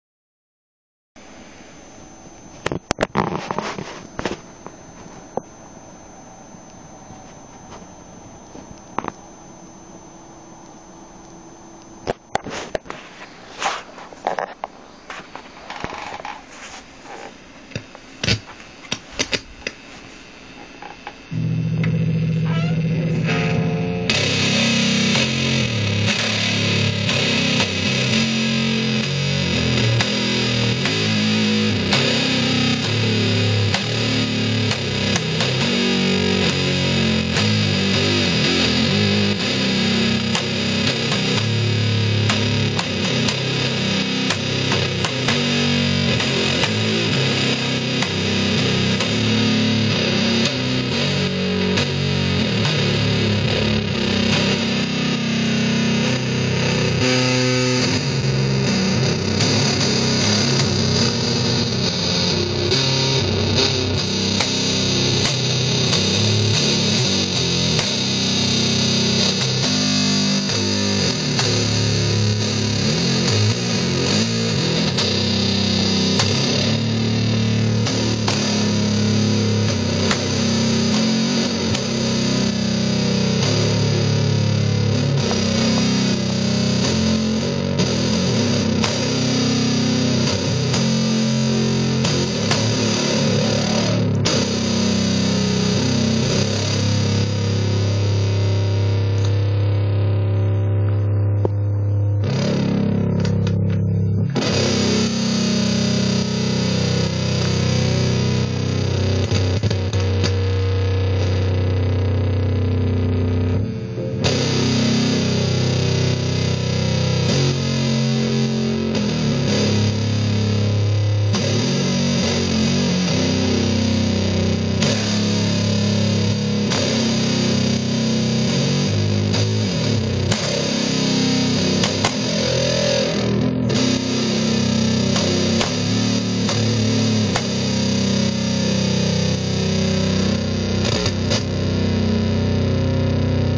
Doomed FX86B DOD Pedal & GX15 "Funeral" Crate Amp
I'm a novice bass player, but I really love the sound I have with this ridiculously simple set up that I use for writing and rehearsing. Oldschool FX86B DOD pedal from the 90's and an old crate practice guitar amp